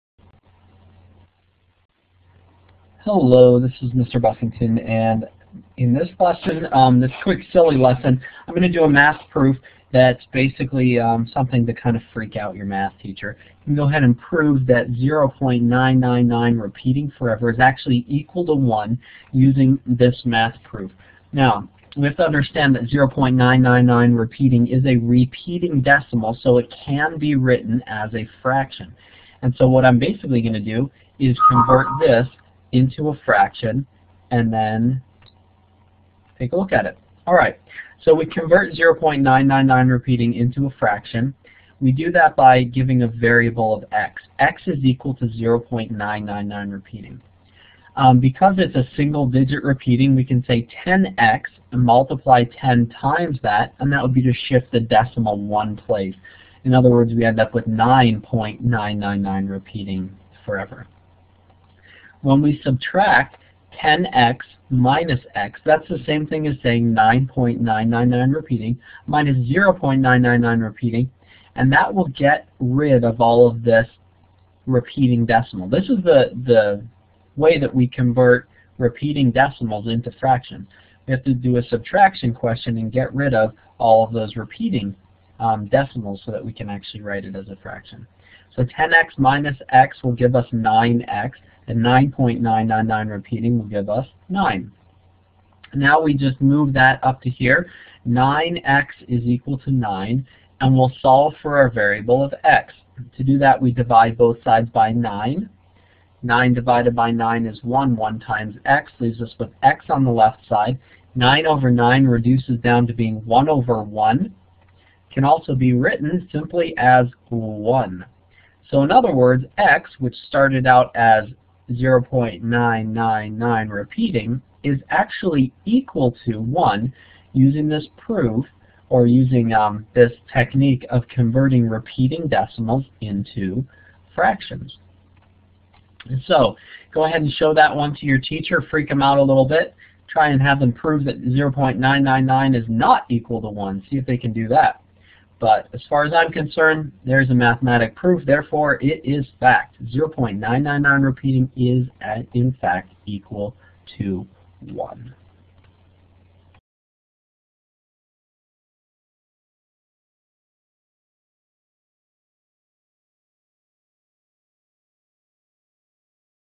Stump your Math Teacher! (Simplifying Math)
A quick lesson that will stump your math teacher, using only math from Algebra 1.  (Converting a repeating decimal into a fraction).  0.999... = 1